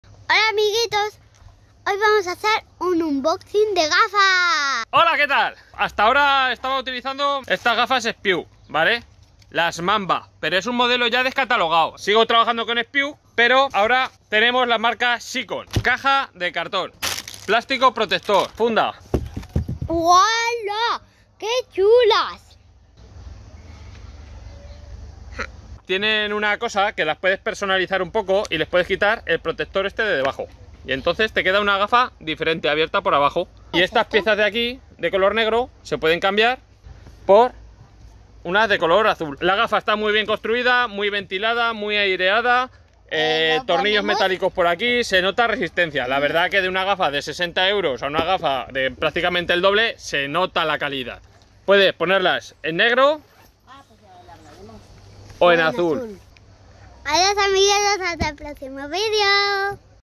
GAFAS SCICON AEROWING UNBOXING
Aunque continuo trabajando con las gafas marca Spiuk añadimos a nuestro catalogo los modelos de SCICON hoy os mostramos el modelo

CONTACTO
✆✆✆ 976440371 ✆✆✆

Ciclos Almozara.
Calle la Almunia nº 20 50003 Zaragoza